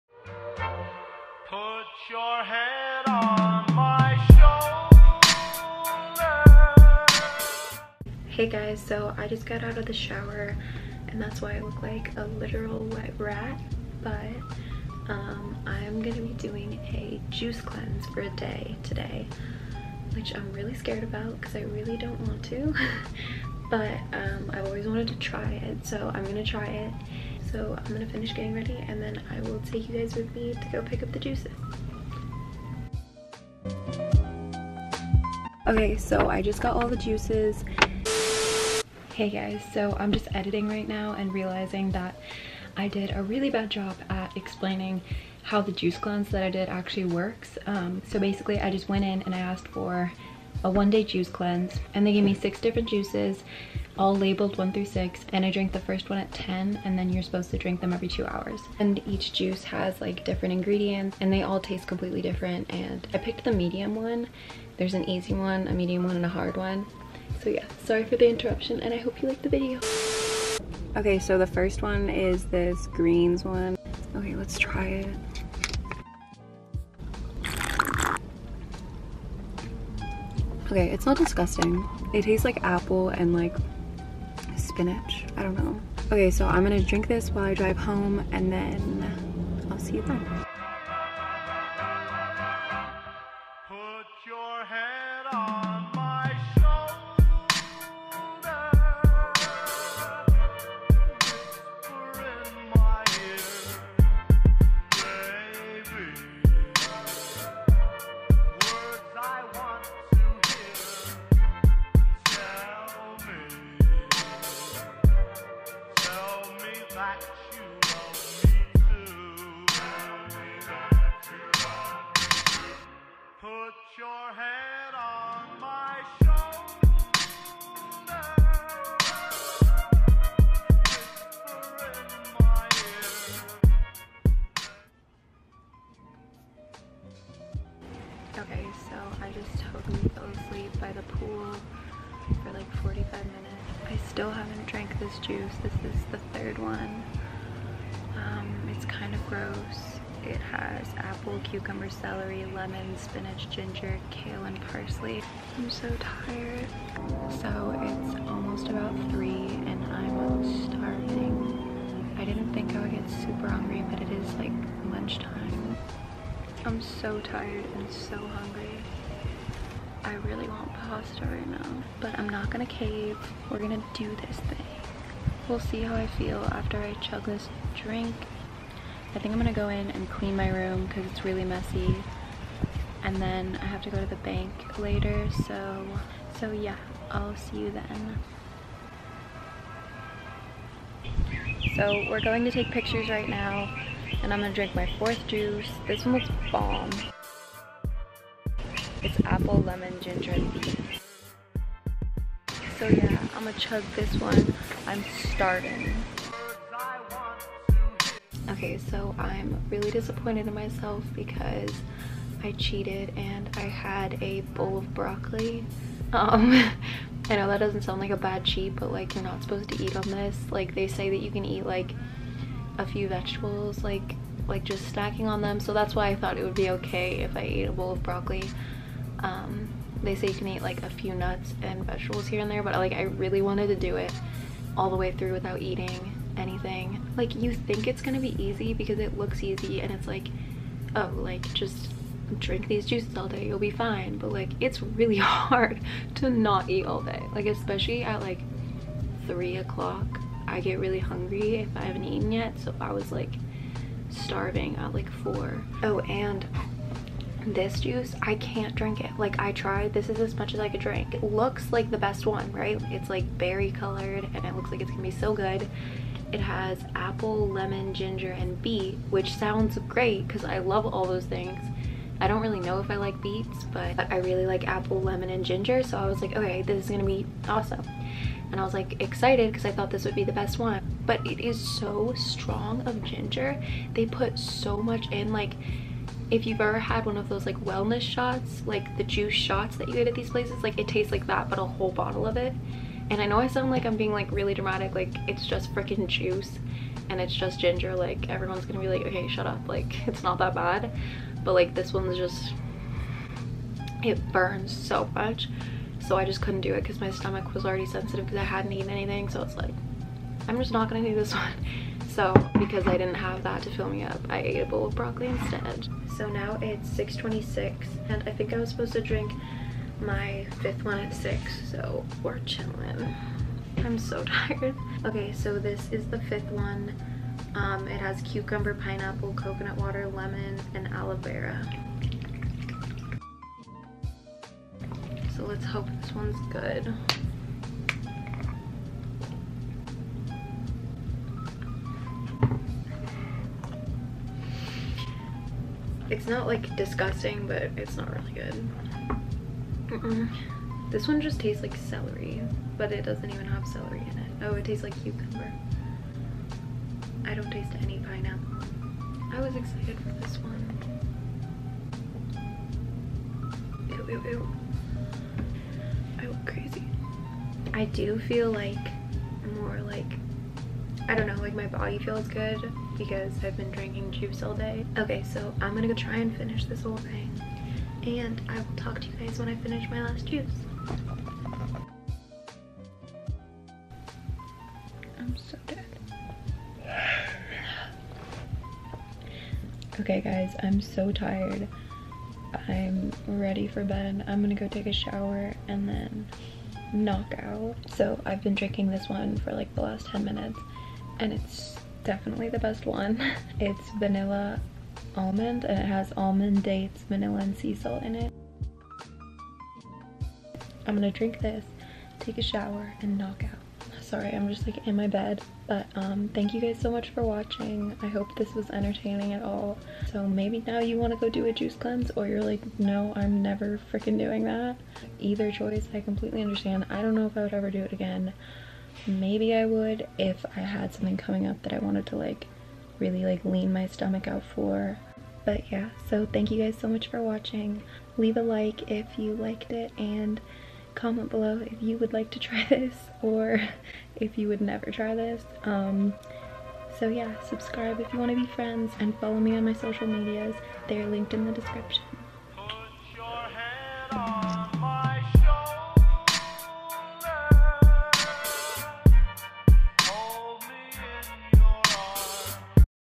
Trying a juice cleanse for a day (hating it)
Hey angels, here's a little video of my trying to do a juice cleanse for a day and realizing how hard it actually is...